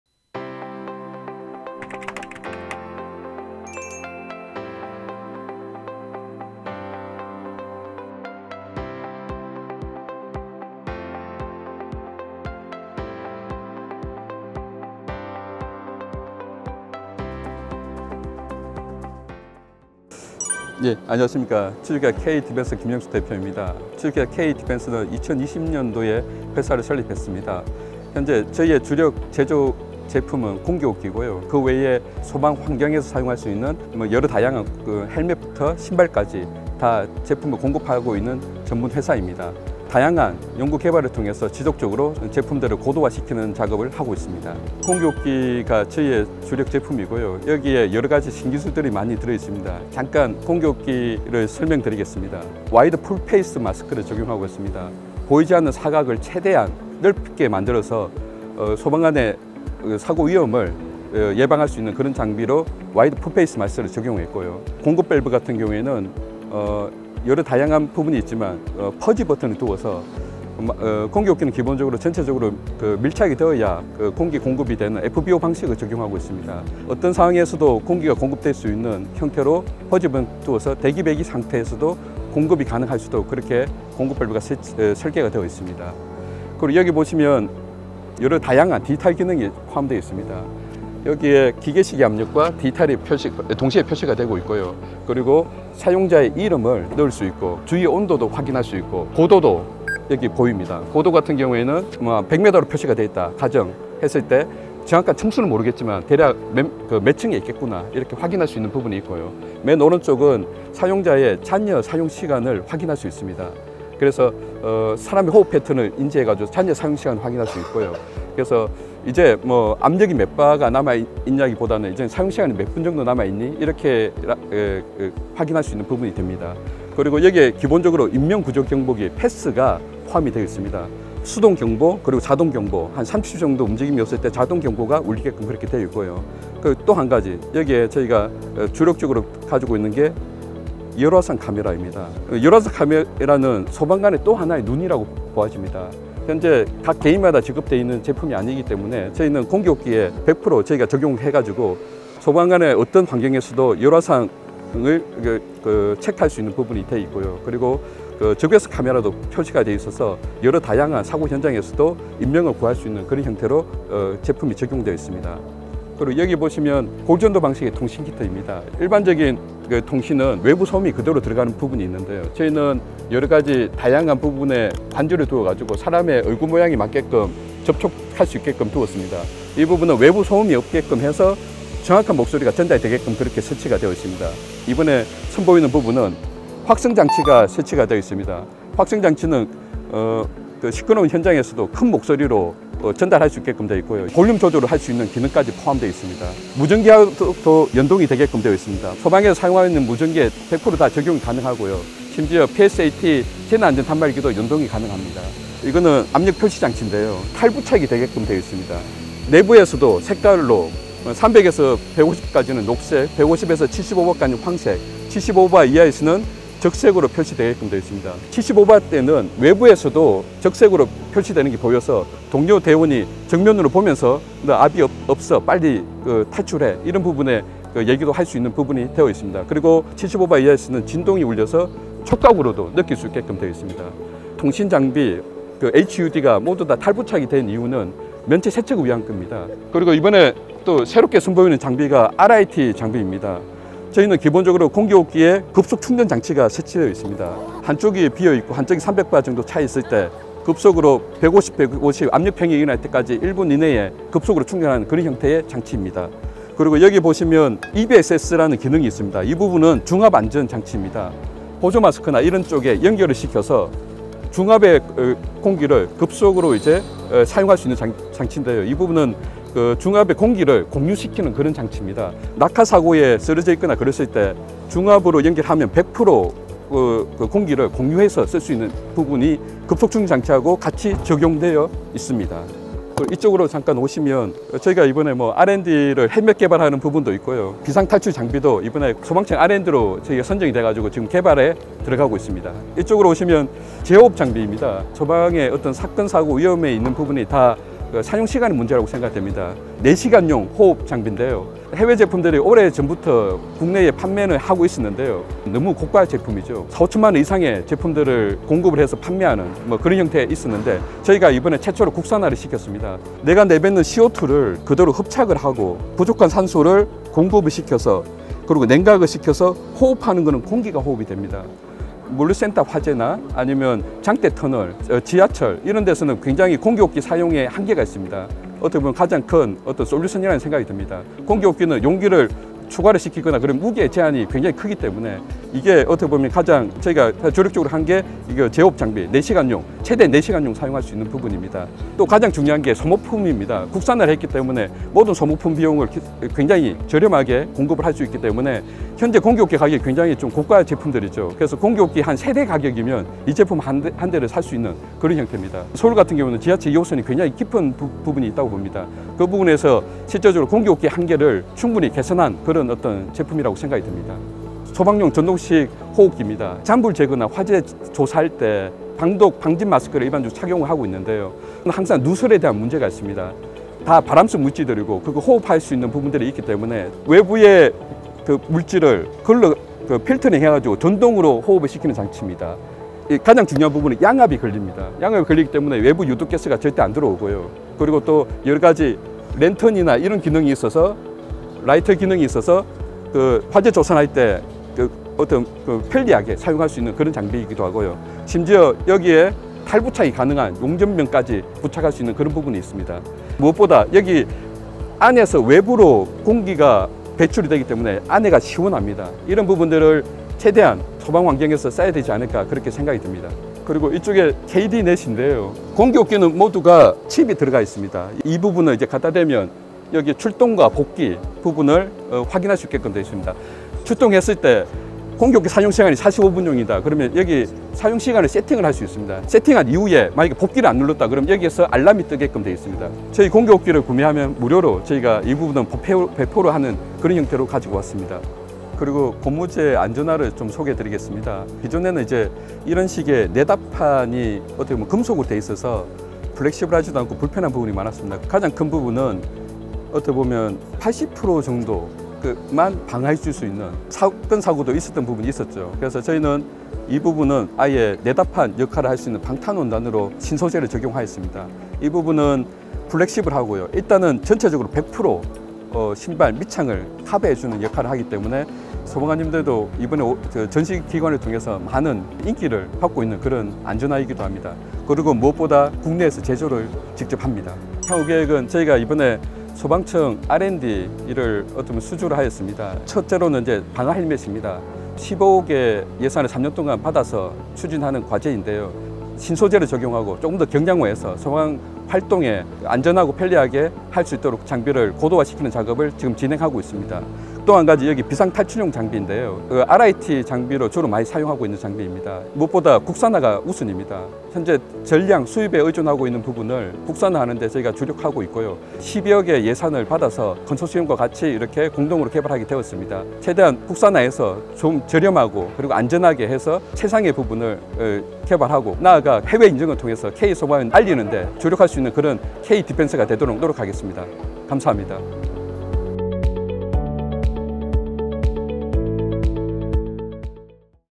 [2024 FireEXPO] ‘IT 융복합 기술로 탄생한 소방용 공기호흡기’ (주)케이디펜스
(주)케이디펜스는 IT 융복합 기술을 소방용 공기호흡기에 접목시키며 개인보호장비 시장에서 두각을 나타내고 있는 기업입니다.

5월 22일부터 24일까지 사흘간 열린 '2024 국제소방안전박람회'에서 케이디펜스는 IT 융복합 스마트 공기호흡기와 RIT 장비ㆍ재호흡장비ㆍ소방용 전동식호흡기ㆍ신소재 적용 고무제 방화신발 등을 선보이며 관심을 끌었습니다.

FPN TV가 케이디펜스 부스를 찾아 기술에 관한 자세한 얘기를 들어봤습니다.